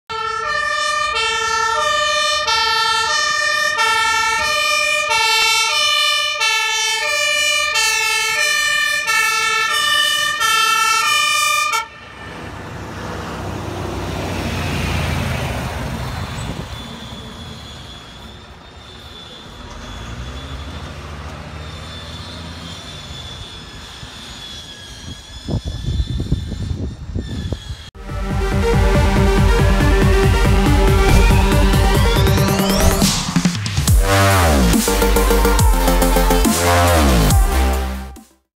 Brandweer TS18-401 met spoed naar een middelbrand bij de Lichtkring in Zwijndrecht
Fire department engine TS18-401 responding to a first alarm fire on the Lichtkring in Zwijndrecht